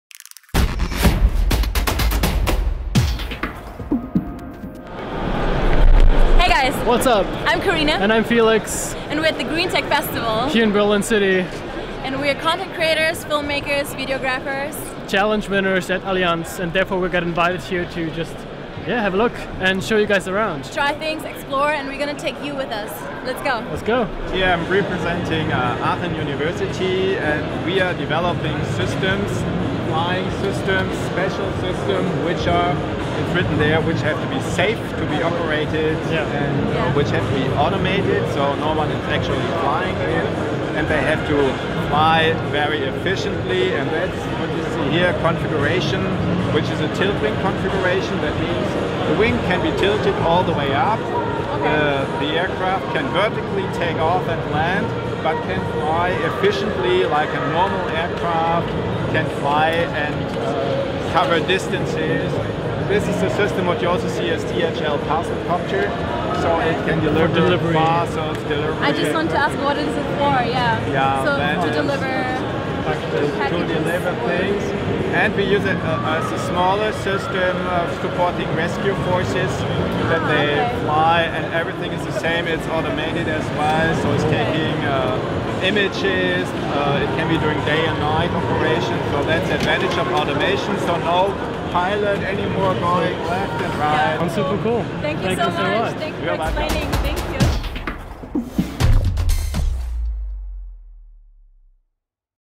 Felix Backhaus & Karina Hilmer - At Greentech in Berlin looking at future of drone delivery.
Greentech Exhibition in Berlin 2019